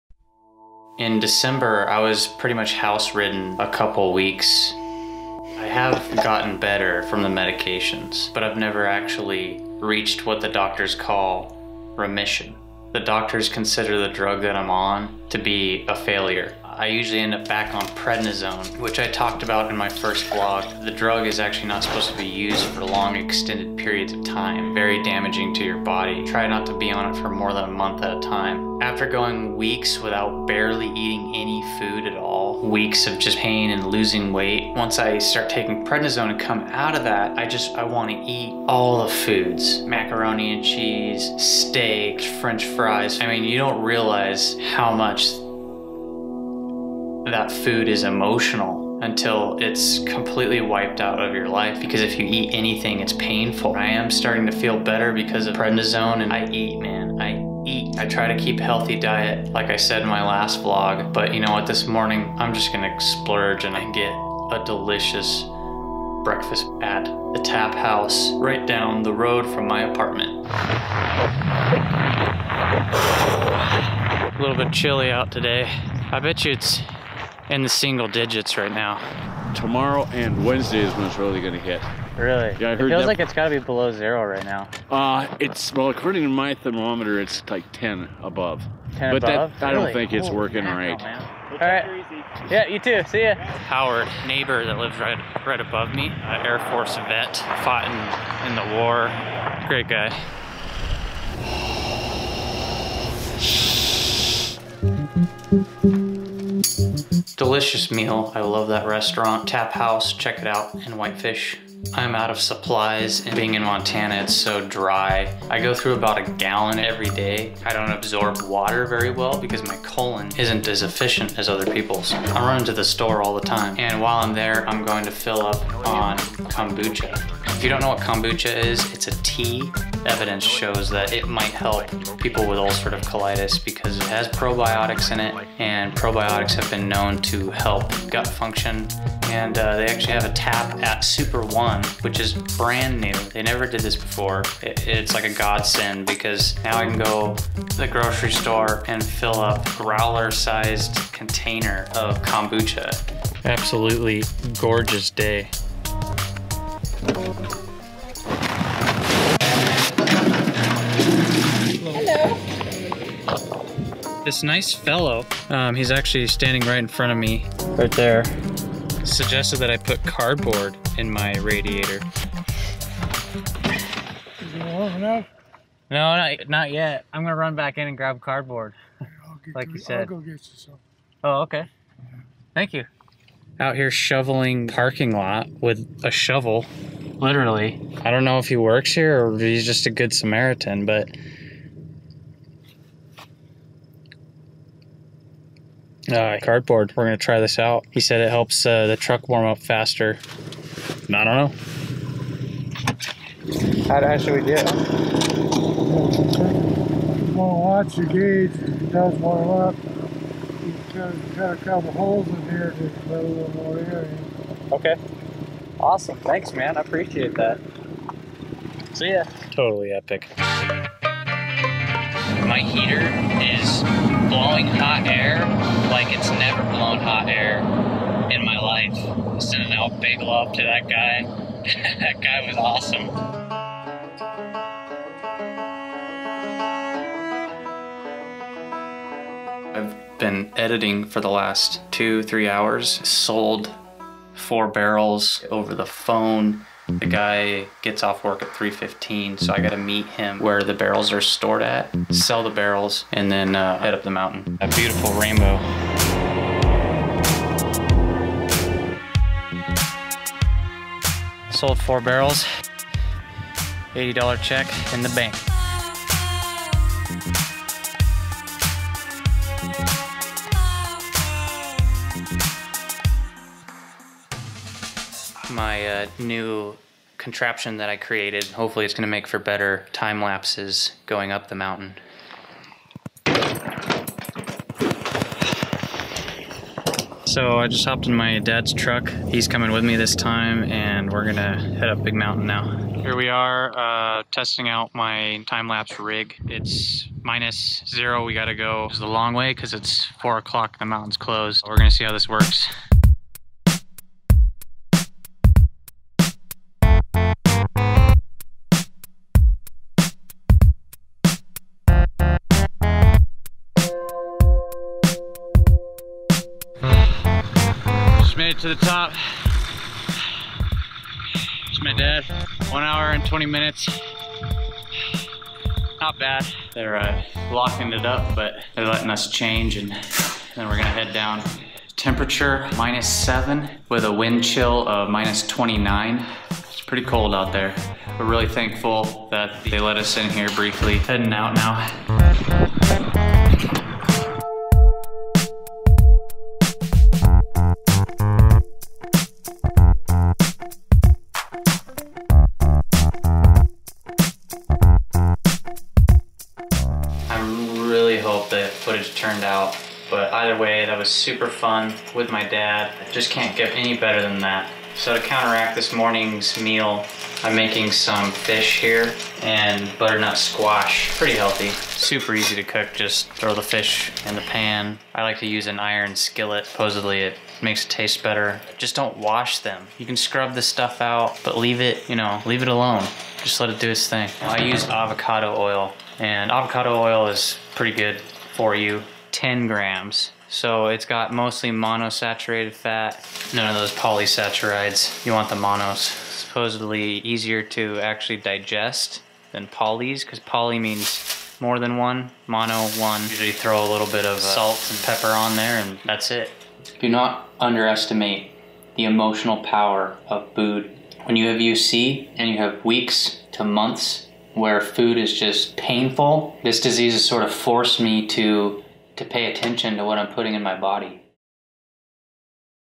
Eating with UC and the Good Samaritan | Vlog8
VLOG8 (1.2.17) I talk about eating with Ulcerative Colitis and prednisone. While on a Kombucha run In sub-zero temperatures I meet a good samaritan along the way. I also test out my new homemade time-lapse rig, with my dad, while skinning up Big Mountain Whitefish Mountain Resort.